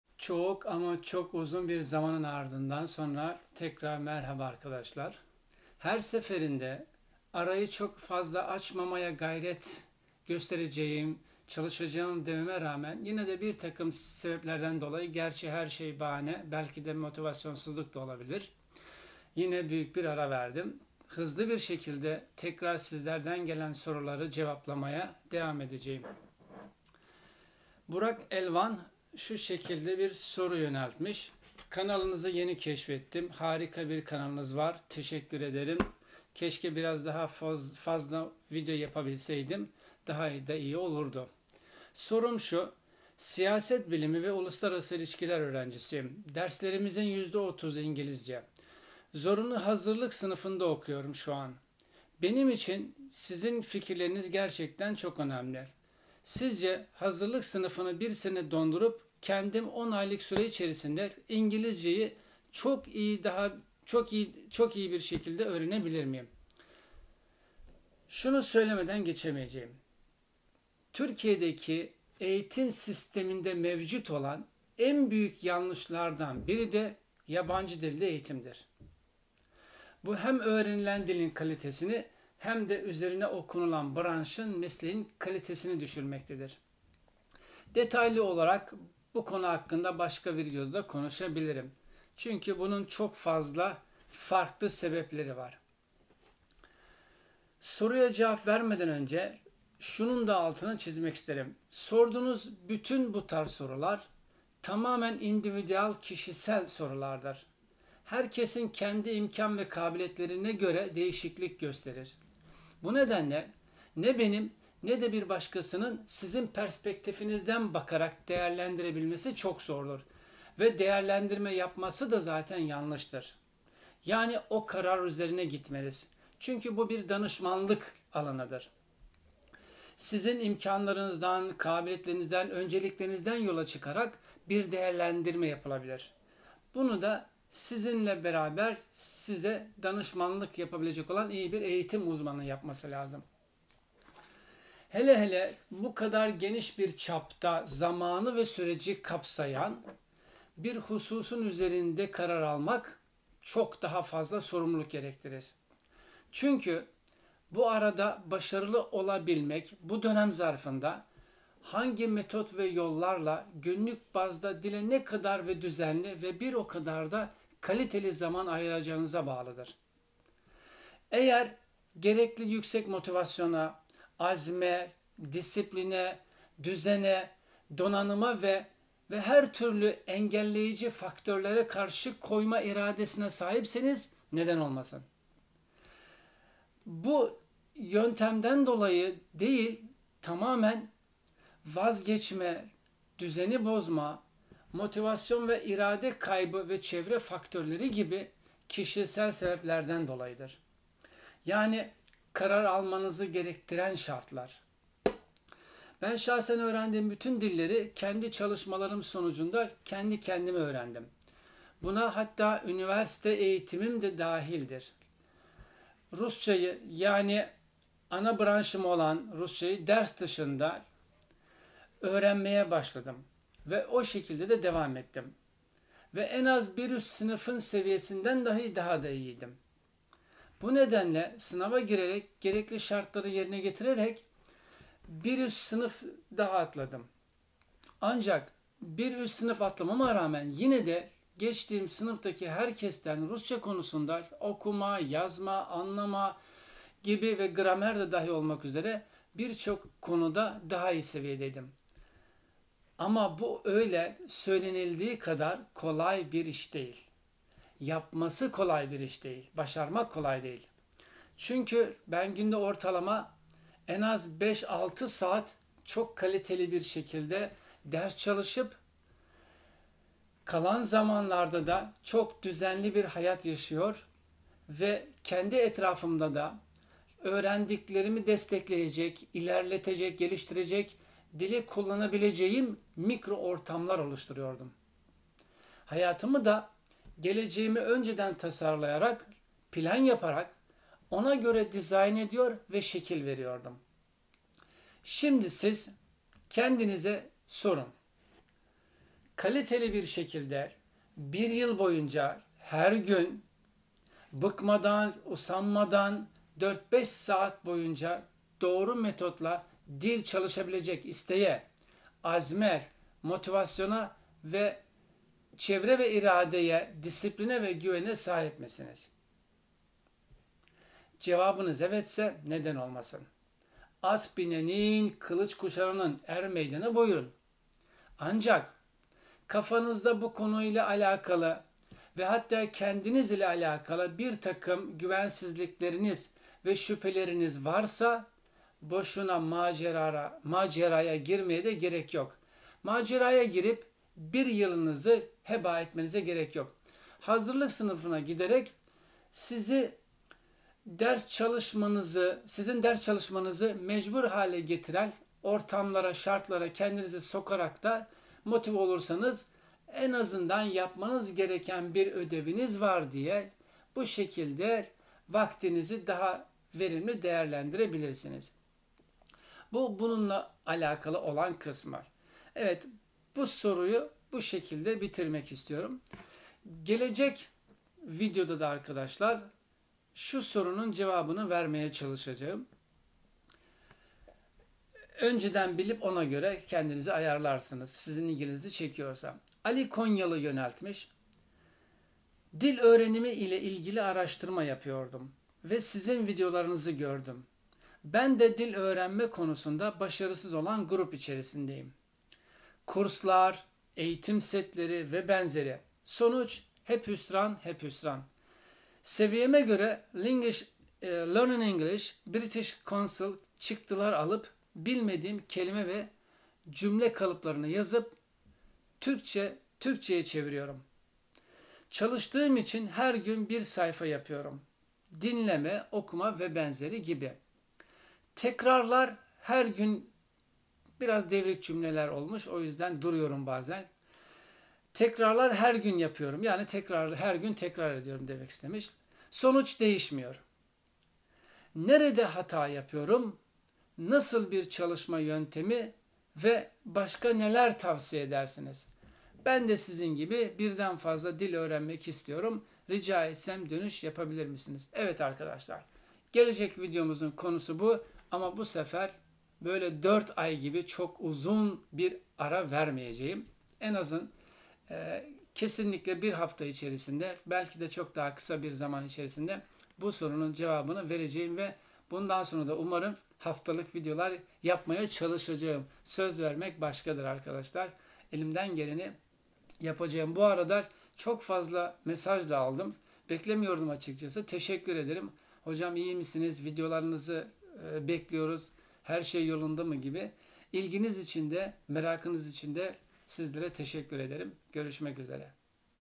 HAZIRLIK SINIFINA GİTMEDEN KENDİ BAŞINA DİL ÖĞRENMEK
yabancıdil ingilizce rusça Üniversite eğitimine hazırlanan öğrencilerin sıklıkla sordukları, söylemesi kolay ancak yapması zor bir konuyu ele aldım bugün. Herkese iyi seyirler.